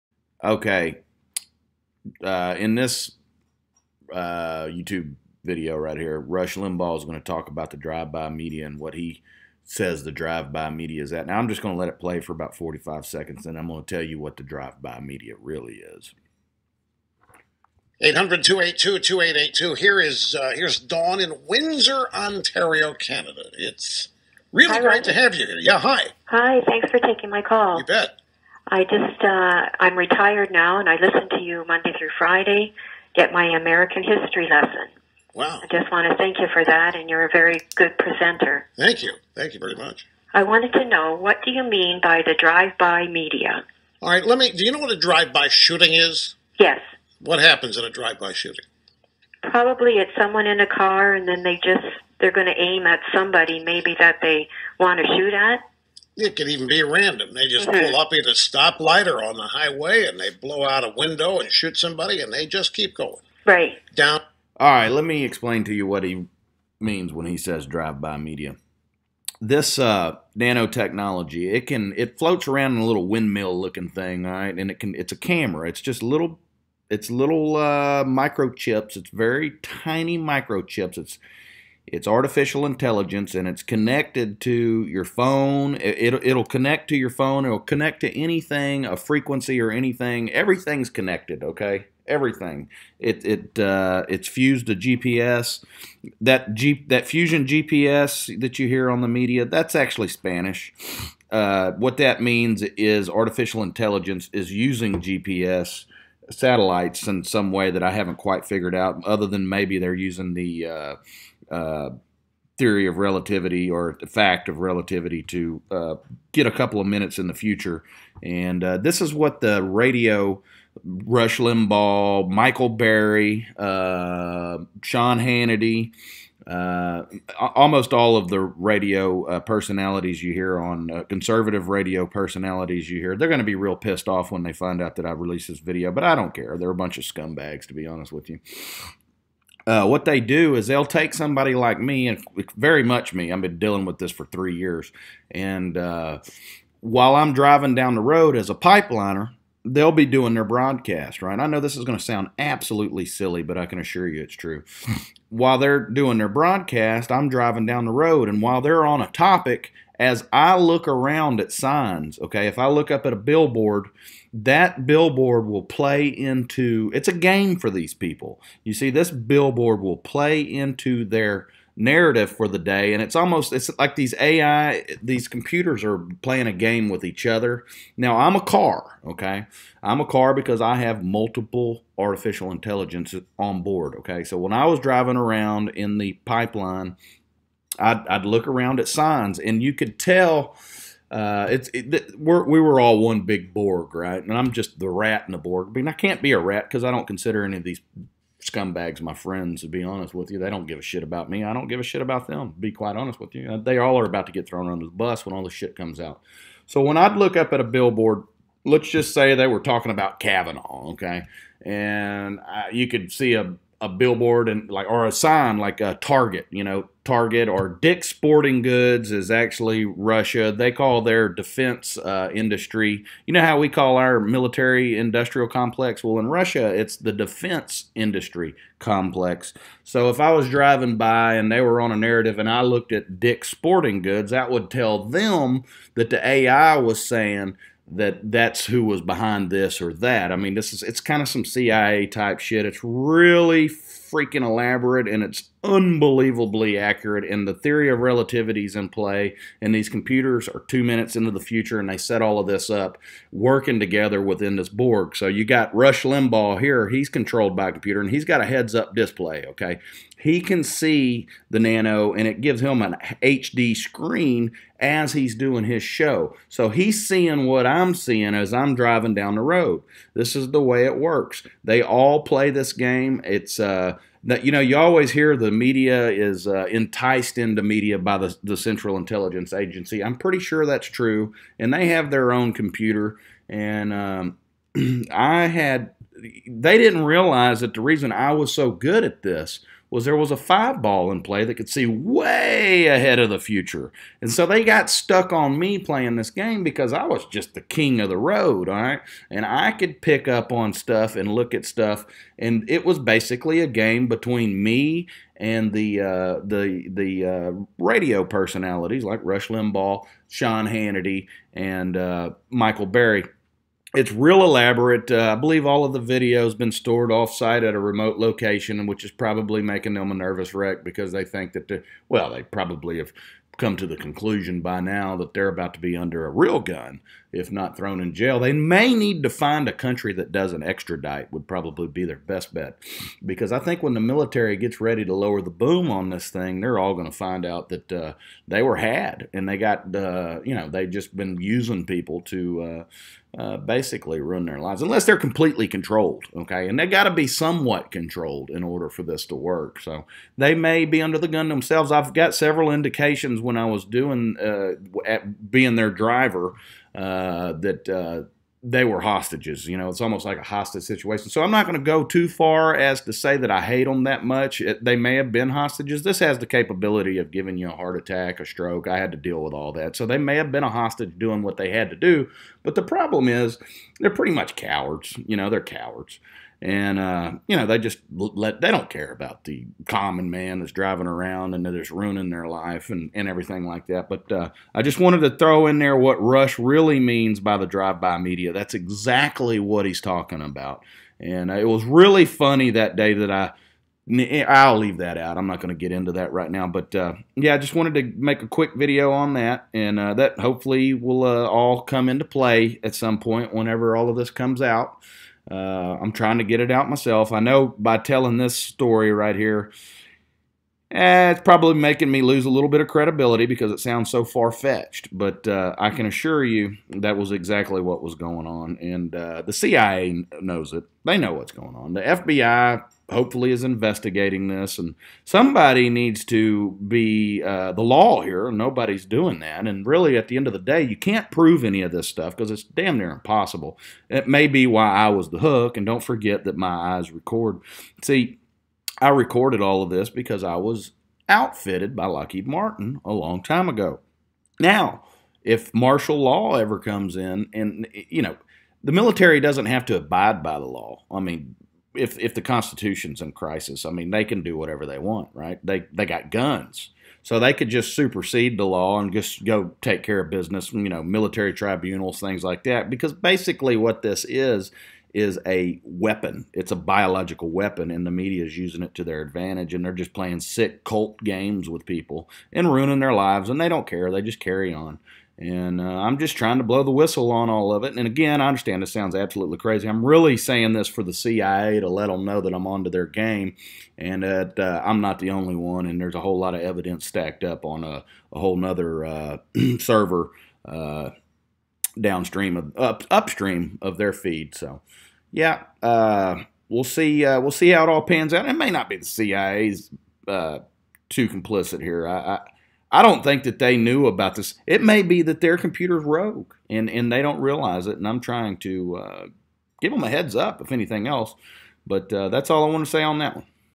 Drive By Media -Explained
How conservative radio hosts use people as slaves in order to play games with each other. Total disregard for humanity.